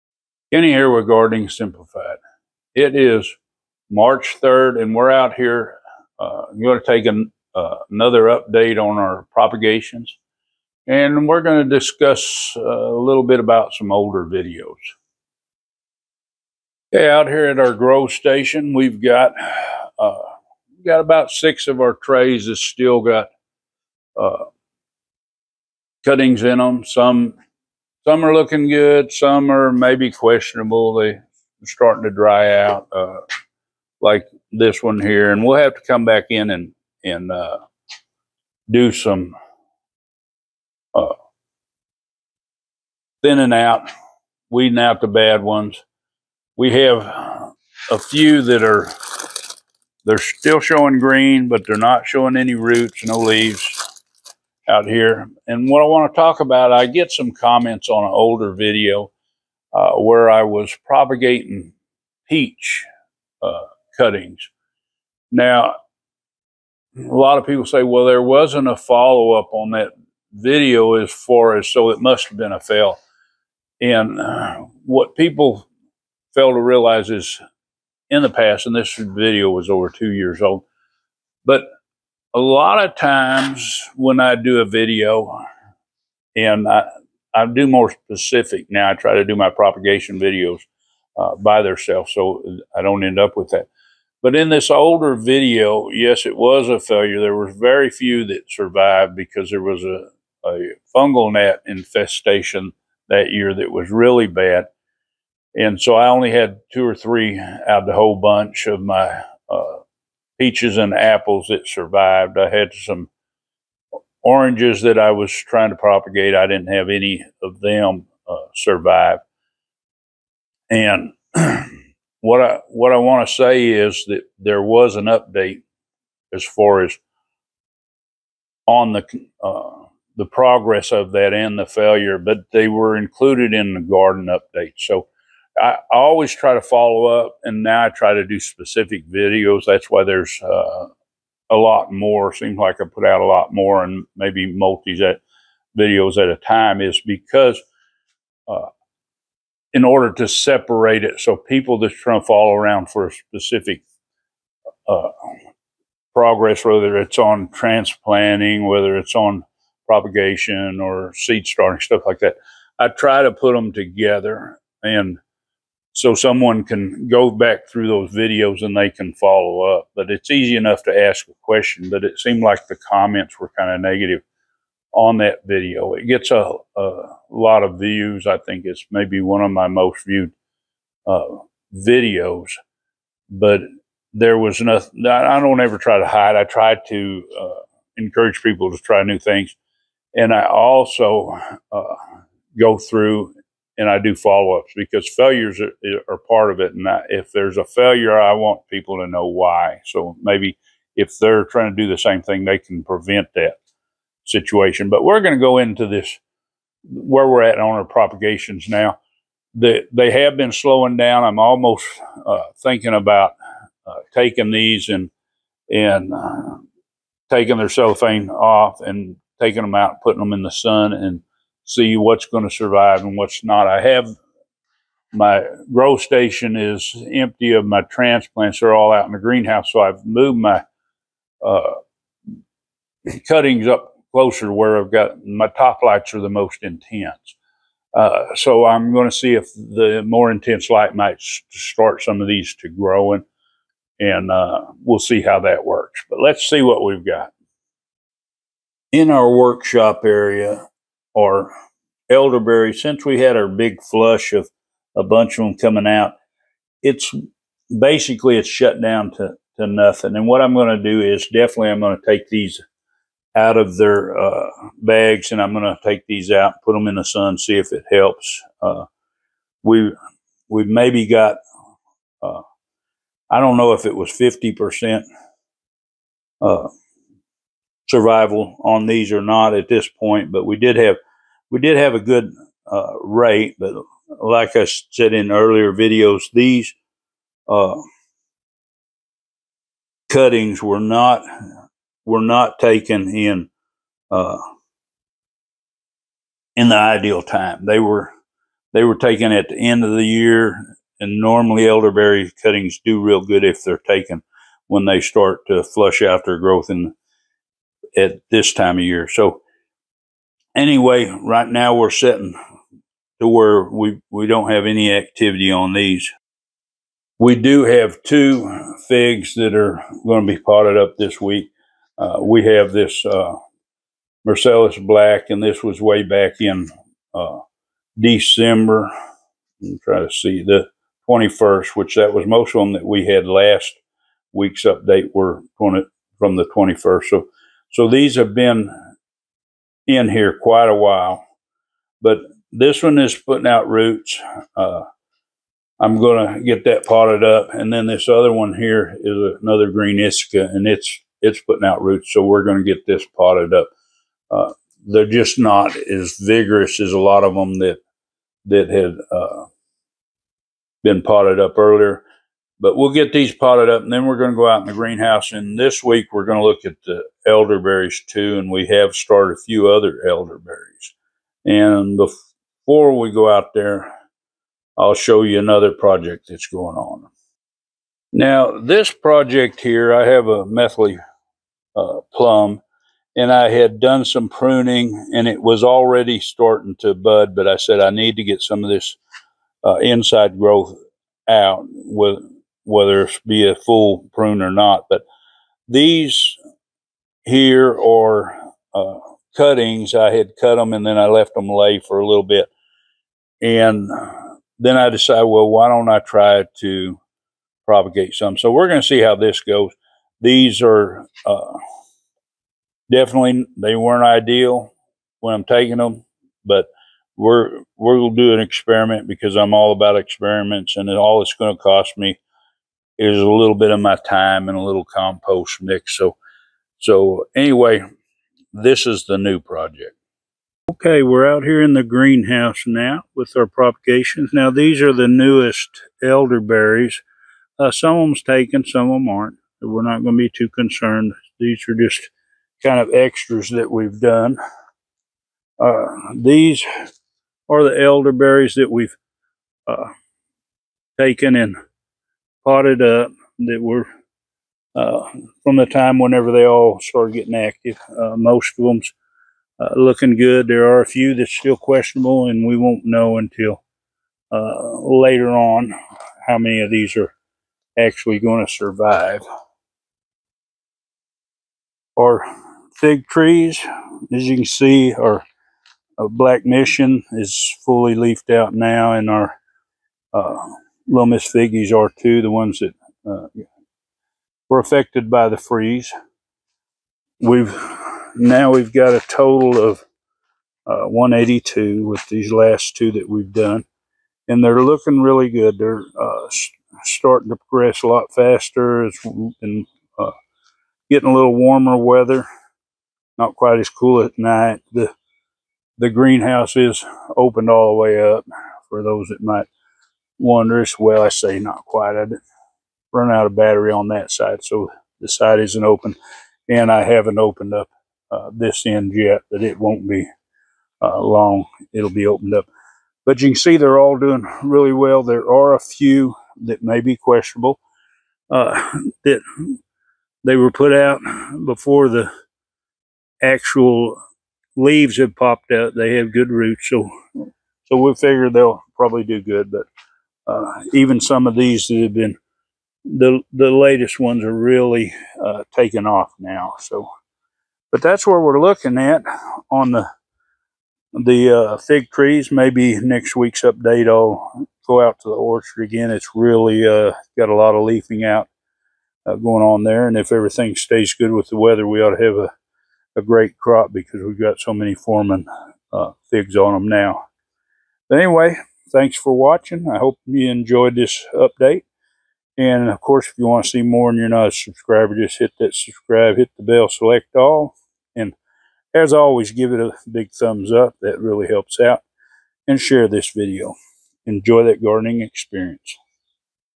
Fig And Elderberry Propagation Update February 3rd
So many fig trees and they are really looking good.